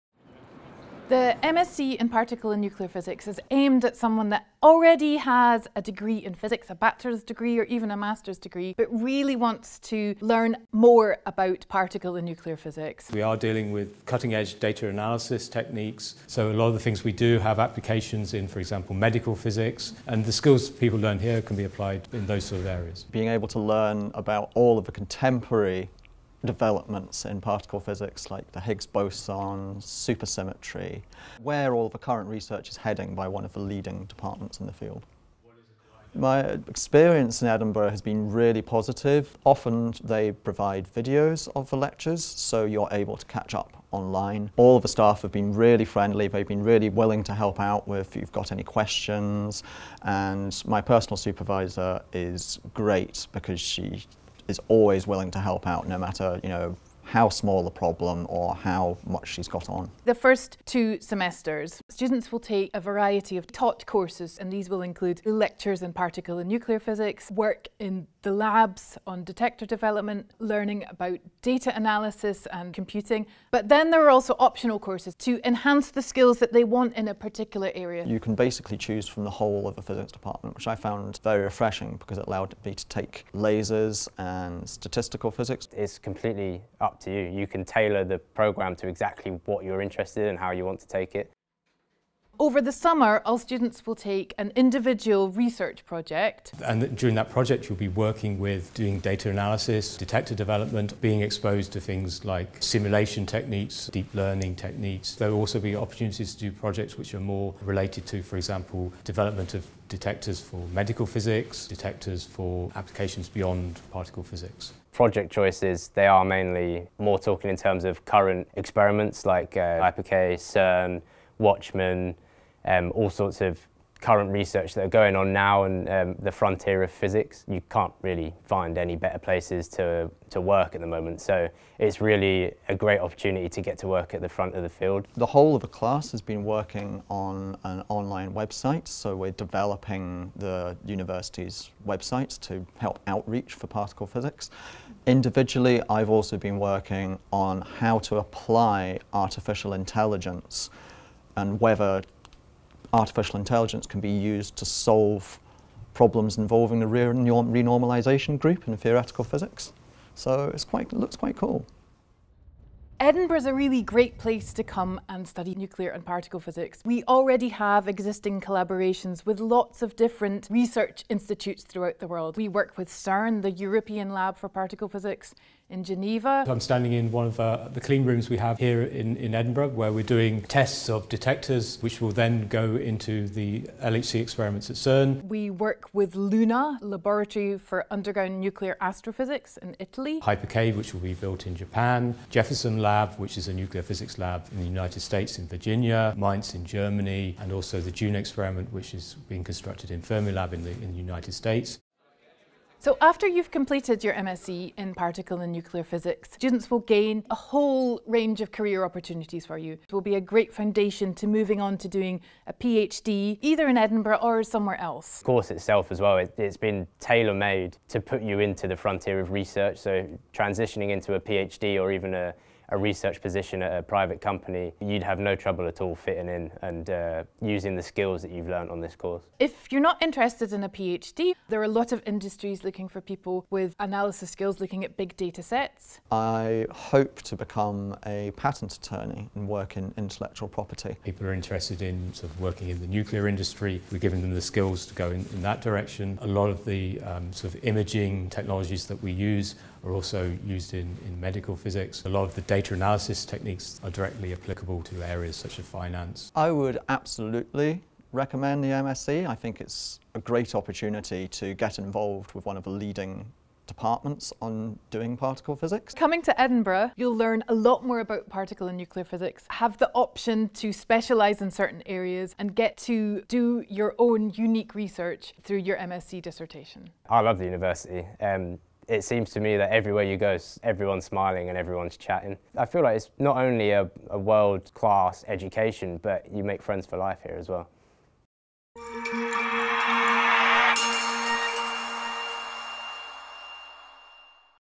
MSc in Particle and Nuclear Physics
Prof Victoria Martin and Dr Matt Needham talk about the skills you will gain, the collaborations you can be involved in, and the career opportunities you can embark on following completion of our MSc in Particle & Nuclear Physics.